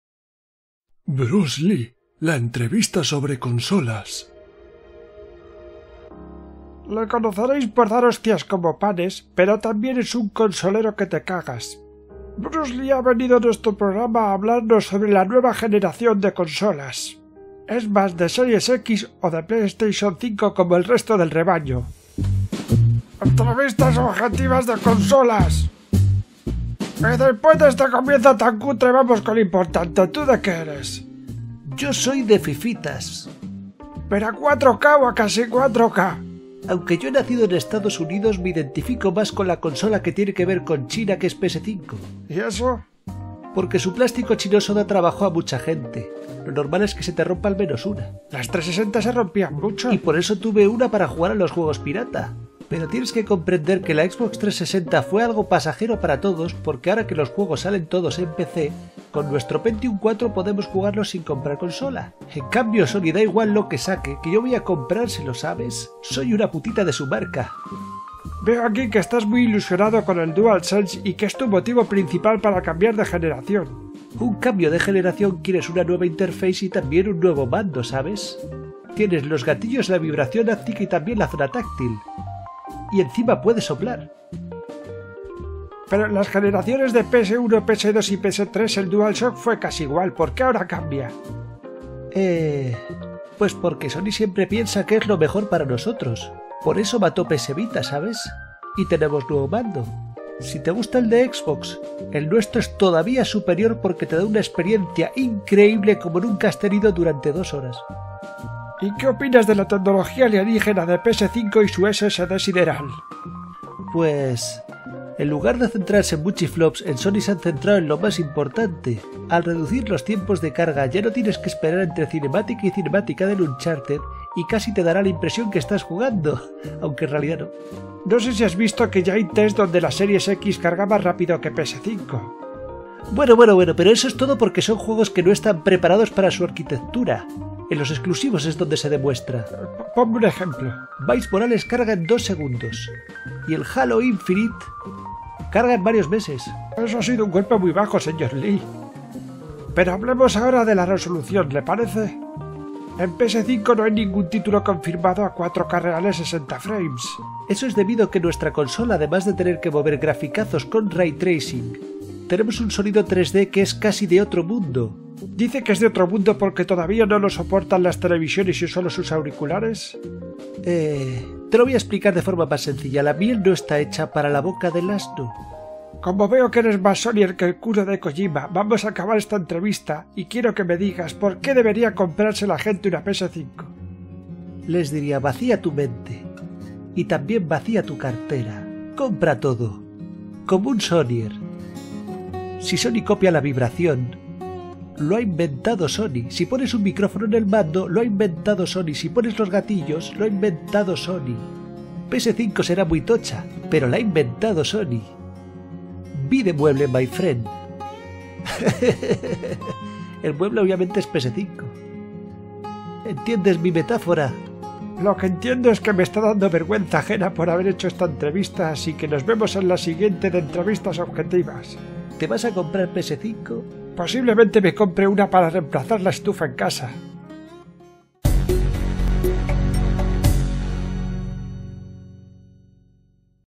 Bruce Lee - La entrevista sobre consolas
En esta verídica y nada inventada entrevista a Bruce Lee, le preguntan sobre la nueva generación de consolas. La mala calidad de la entrevista es porque no se grabó con un iPhone, pero es parte de una entrevista inédita que le hicieron en Albacete hace unos meses. En realidad no está muerto, vive en un pueblecito perdido de la mano de dios donde se gana la vida fabricando aceite.